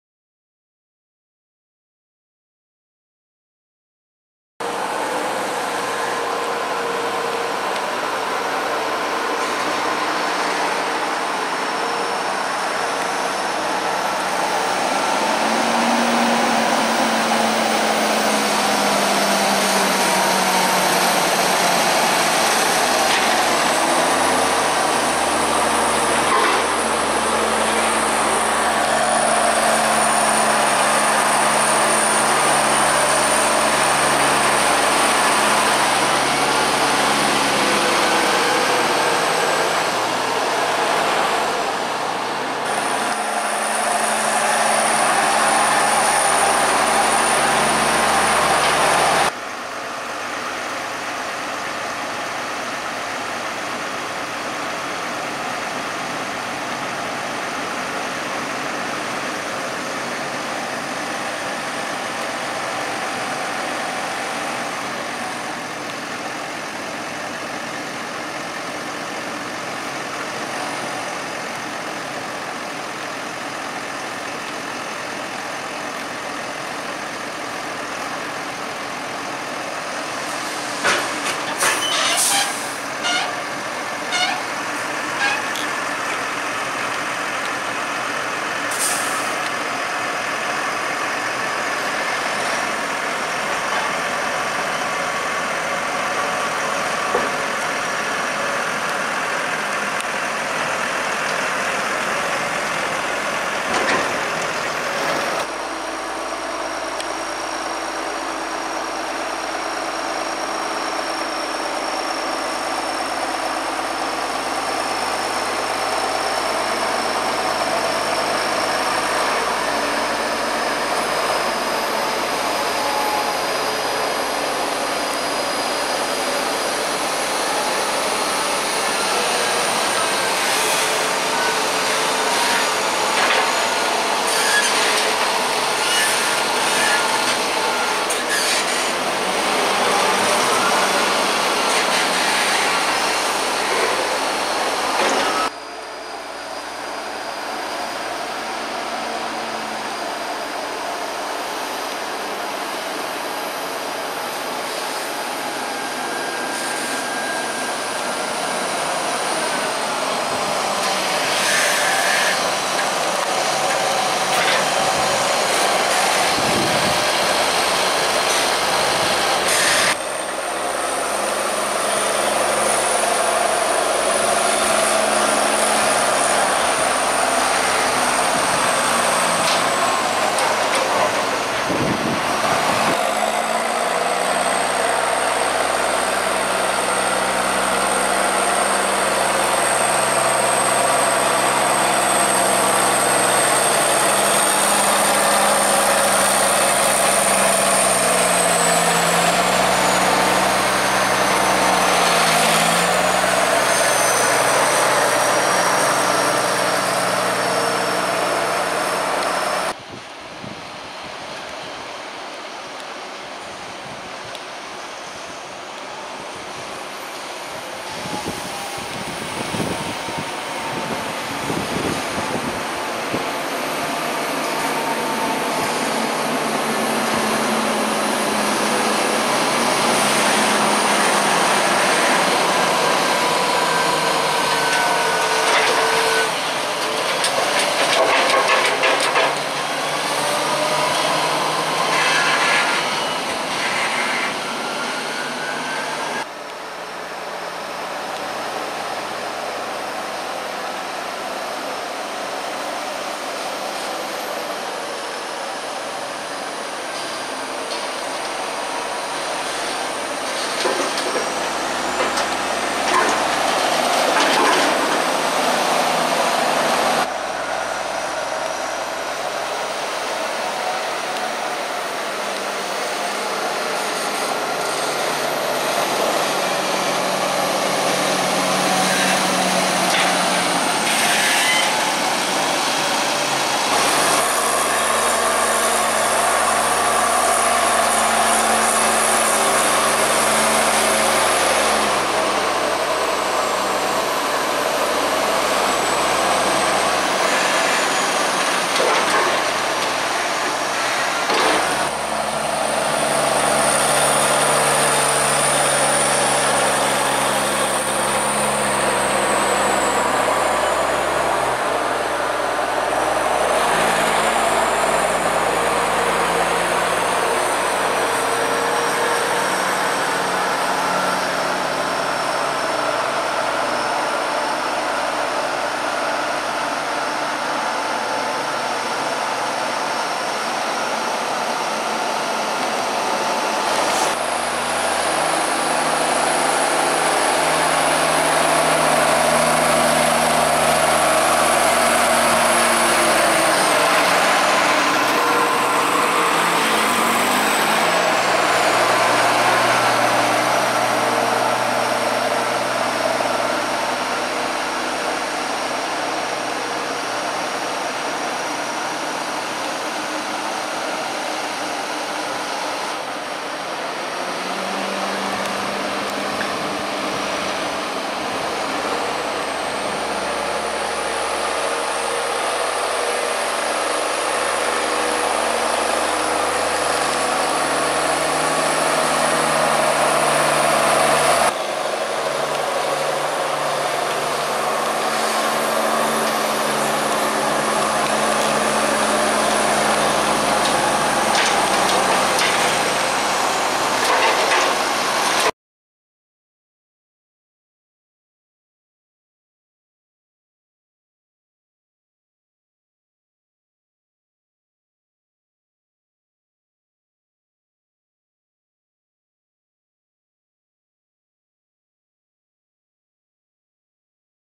Maize Harvest 2022 - Buckraking the pit with Fendt 724 & Albutt plus Case IH MX
Taken back in September. One of the last Maize videos for 2022, here's some action from in the pit having seen the action in the field, ( video of the chopping at this farm in the Cards section). Farms new to them Fendt 724 Vario buckraking the maize with Albutt buckrake, originally on the farms Case Puma 240 which can be seen on the channel buckraking grass. Also assisted by farms Case IH MX135 rolling the pit and making quite a sound. With thanks to all involved and Andrew for the tip. Filmed in Hampshire, UK. Watch in HD for best quality.